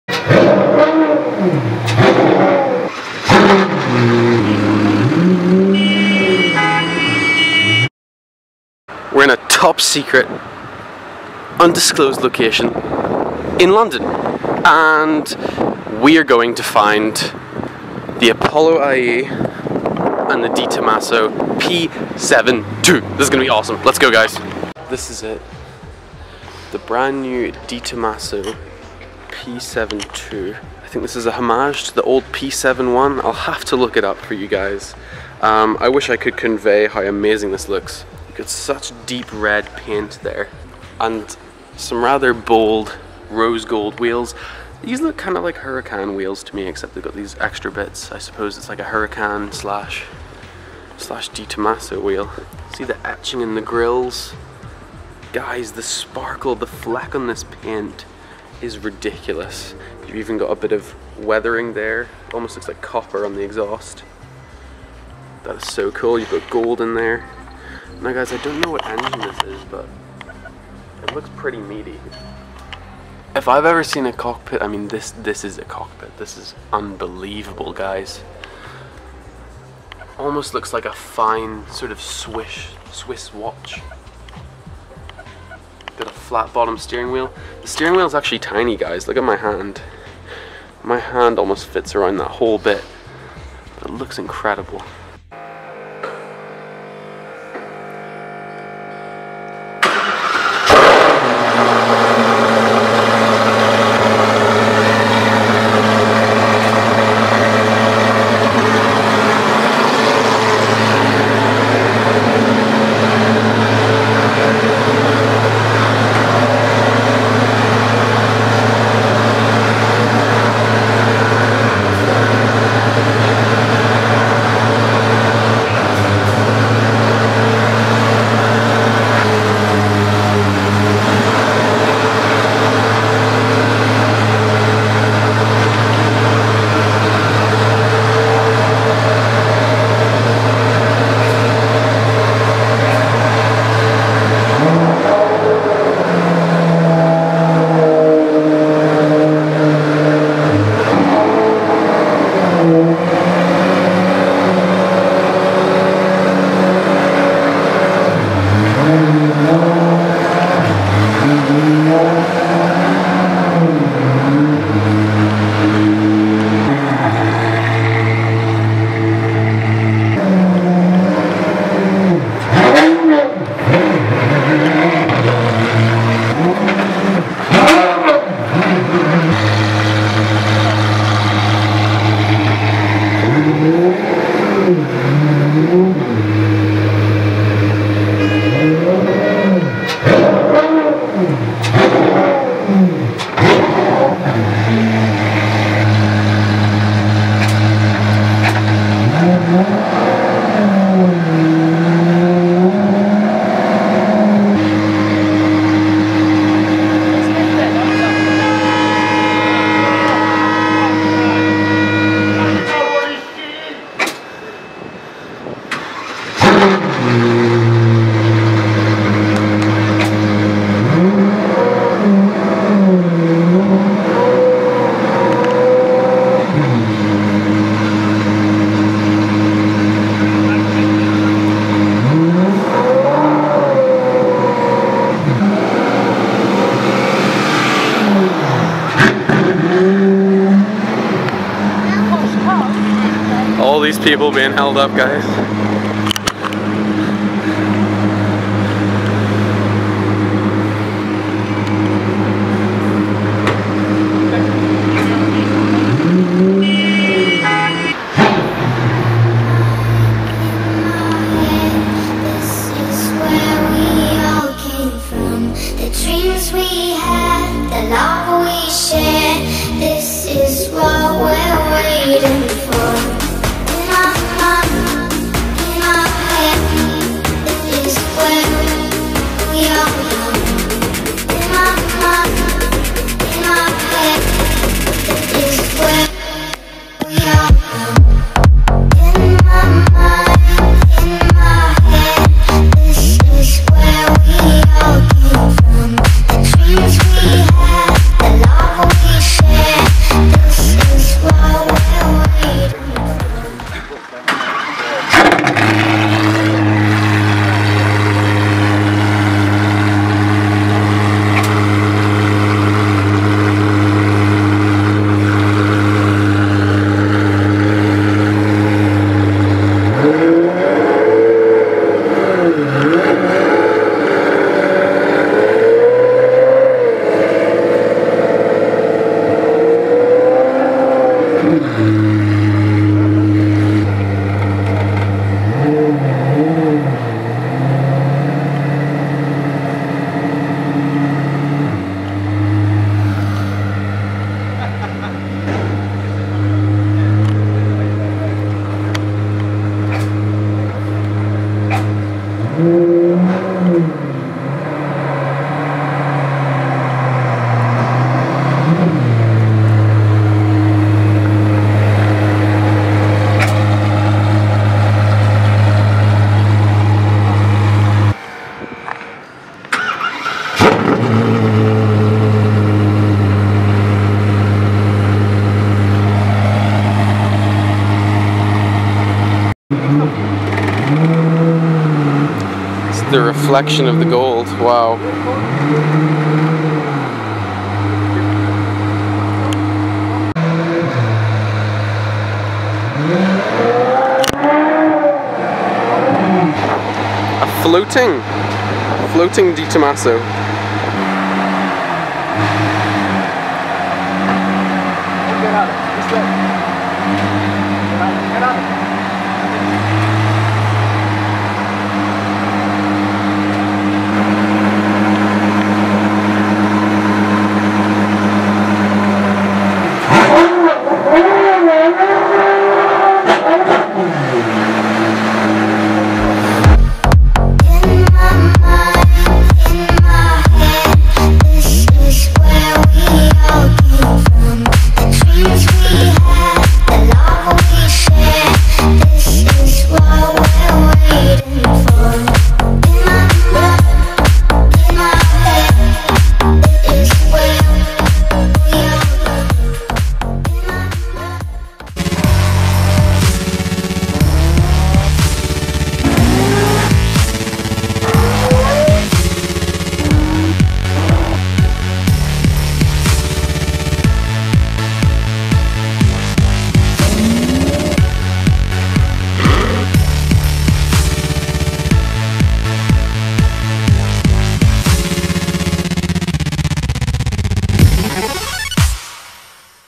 First DeTomaso P72 on Public Roads brings traffic to a standstill *Deafening V12 Sound!*
In this video you'll get to see the first ever footage of the brand new DeTomaso P72  driving on the street. The car was only revealed last week at Goodwood in the UK and you'll see that its low front nose made it difficult to get into a driveway, much to the quite comical annoyance of the people of London. This must be one of the most beautiful cars to ever be created and pays homage to a lot of 1960s Le Mans cars.